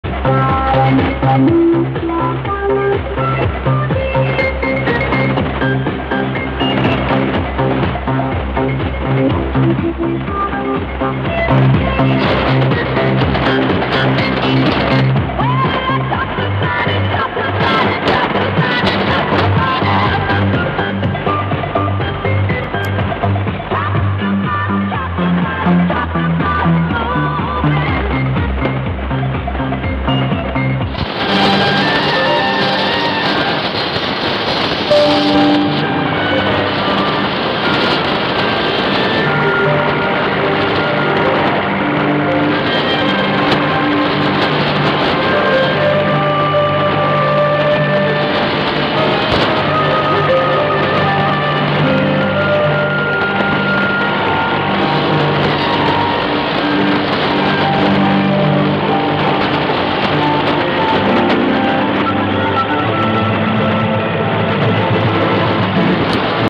2903201514123 German stations on 75 m band
22:28 UTC, 3985 kHz - Deutsches Radio 700, Kall-Krekei (1 kW) and 3995 kHz - HCJB Deutsch, Weenermoor (1,5 kW).
Received in Moscow region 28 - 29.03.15 on Degen 1103, built-in whip antenna.